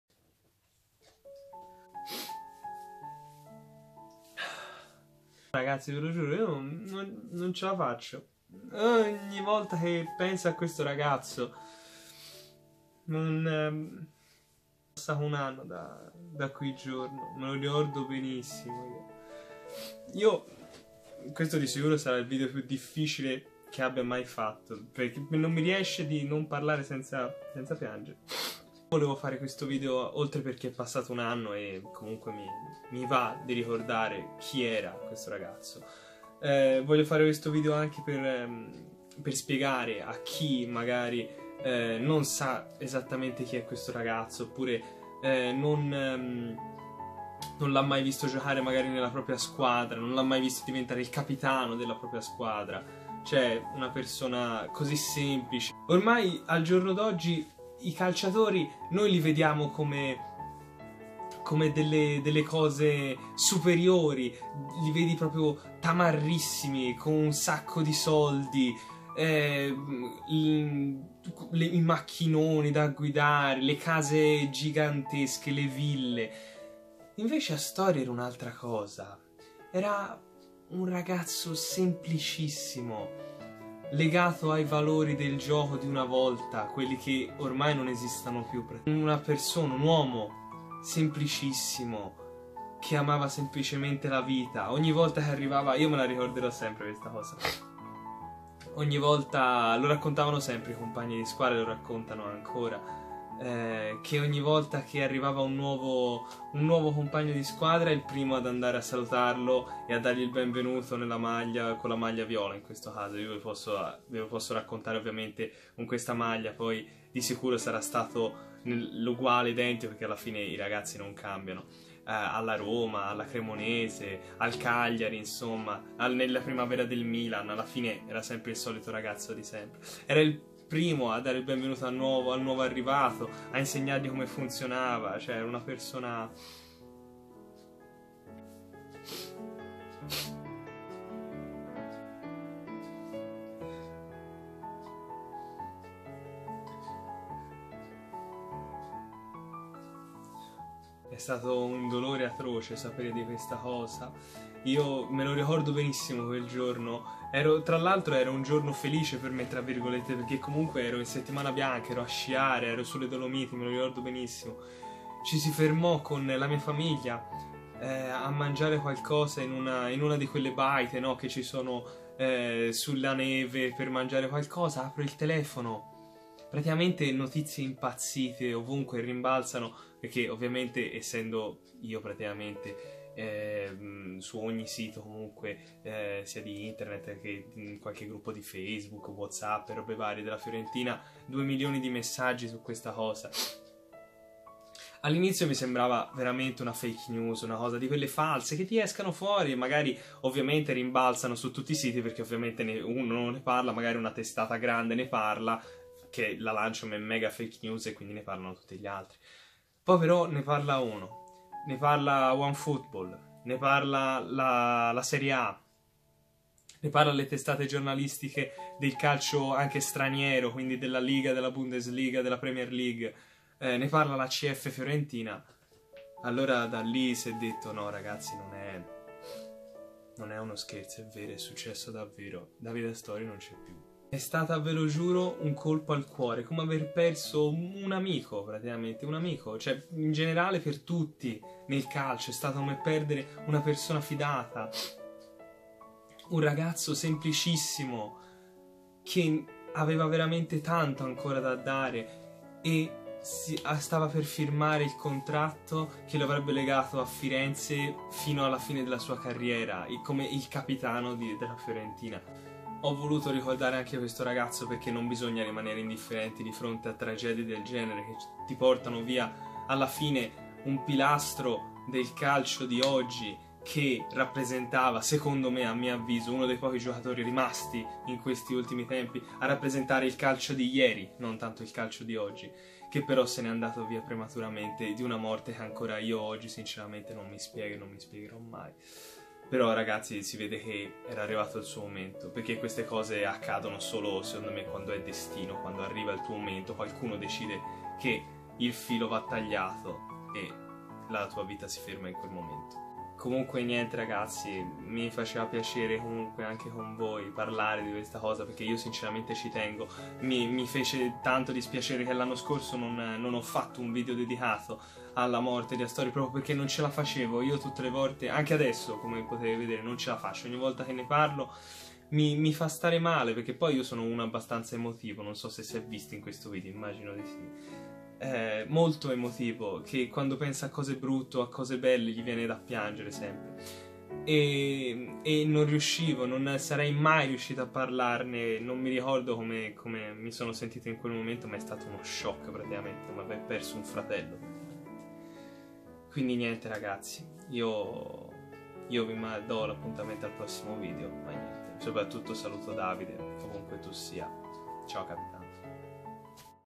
UN ANNO SENZA DAVIDE ASTORI.....
Ragazzi vi posso assicurare che questo è stato in assoluto il video più difficile da registrare che abbia mai fatto...
Un mio piccolo tributo per un giocatore. un uomo, il capitano!